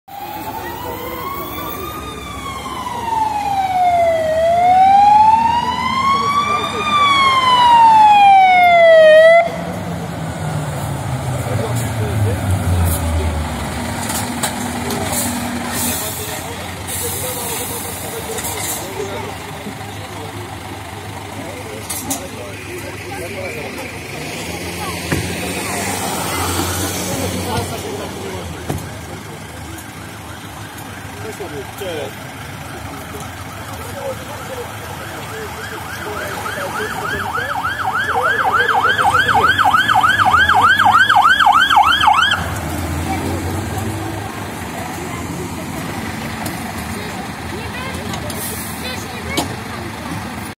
Dojazd 352[S]21 i 352[S]43 do zderzenia osobówek
Kolizja na skrzyżowaniu ulicy Obrońców Westerplatte z Grochowskiego w Częstochowie. Kierujący Audi, z uwagi na wysokie krzewy, wyjeżdżając z osiedlowej uliczki wymusił na prawidłowo jadącym Oplu. Nikt nie został poszkodowany.

352[S]21 GBA 2,7/25 Renault Range D14 - JRG 2
352[S]43 SRt Scania P370 - JRG 2